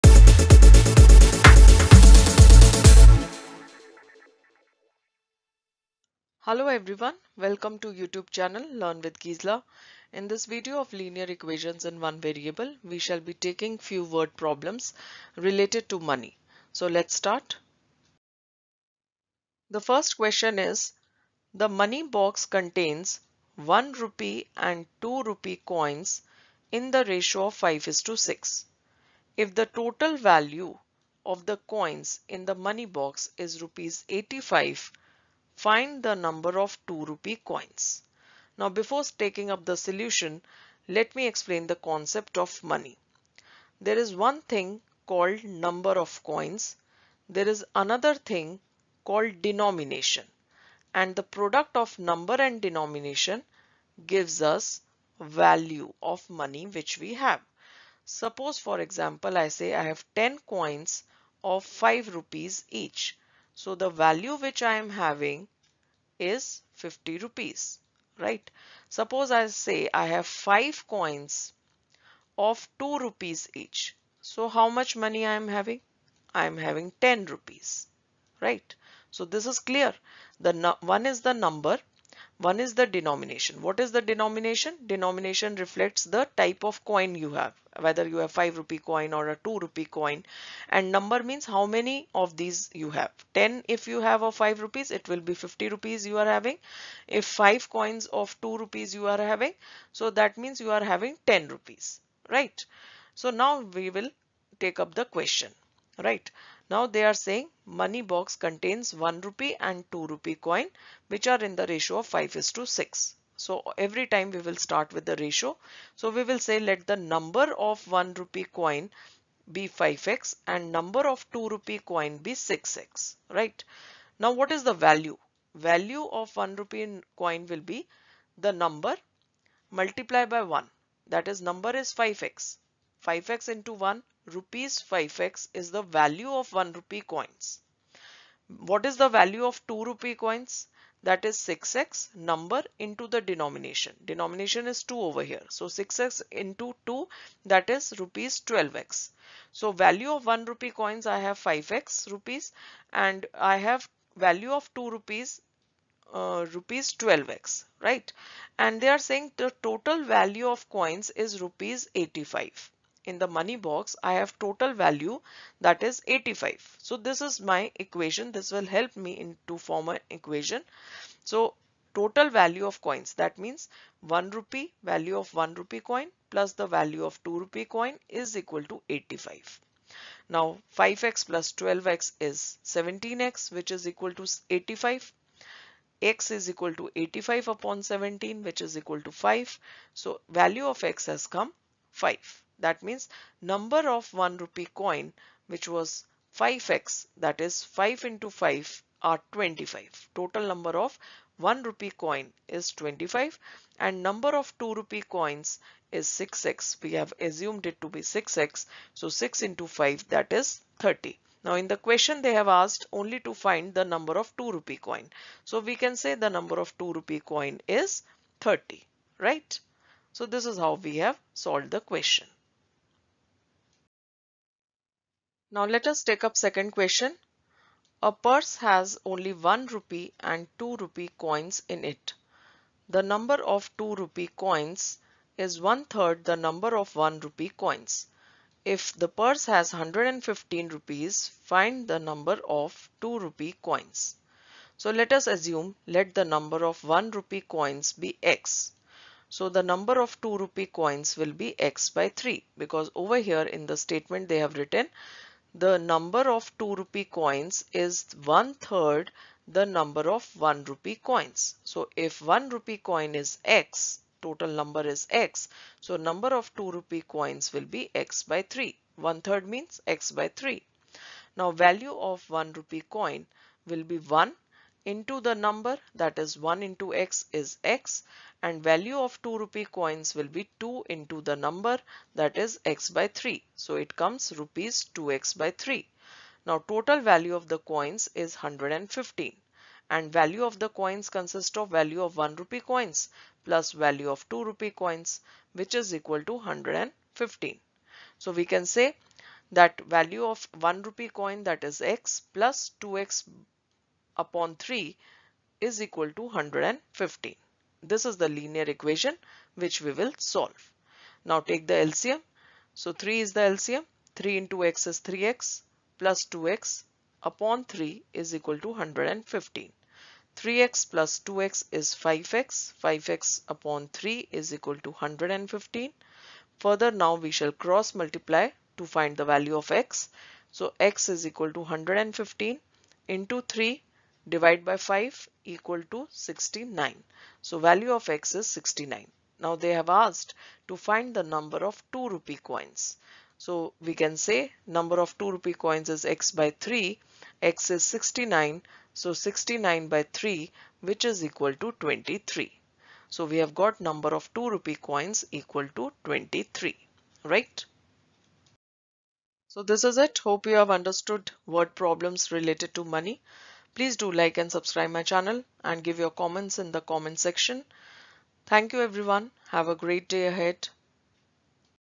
Word Problem related to No. of coins/money- Linear Equations in one variable-SQ-11 DAV Math -Class 8
CBSE Class 8 Math - Linear Equations in one variable-Solved Questions -11
Word Problems Related to number of coins / money - DAV MATH

Q1. The money box contains one rupee and two rupee coins in the ratio 5 : 6. If the total value of the coins in the money is ₹.85 find the number of two- rupee  coins.
Q2. A purse has only one- rupee and two- rupee coins in it. The number of two- rupee coins is one third the number of one-rupee coins. If the purse has ₹ 115 find the number of two-rupee coins.

An equation in which the maximum power of a variable is one (i.e degree is 1) is called a linear equation. 𝐞.𝐠. 𝟓𝒙−𝟗=𝟎
The linear equation which consist of only one variable is called linear equation in one variable.
The standard form of linear equation in one variable is 𝐩𝒙+𝒒=𝒐,
where, 𝒑,𝒒 are real numbers and 𝒑≠ 𝟎 .
Any value of the variable that satisfies the given equation is called its solution or root of the equation.
𝑻𝒉𝒊𝒔 𝒗𝒂𝒍𝒖𝒆 𝒐𝒇 𝐱 =  (−𝒒)/𝒑  𝒊𝒔 𝒕𝒉𝒆 𝒔𝒐𝒍𝒖𝒕𝒊𝒐𝒏 𝒐𝒇 𝒕𝒉𝒆 𝒍𝒊𝒏𝒆𝒂𝒓 𝒆𝒒𝒖𝒂𝒕𝒊𝒐𝒏
To solve (𝒂𝒙+𝒃)/(𝒄𝒙+𝒅)=𝒌, we are going to use cross multiplication method to reduce it to standard form.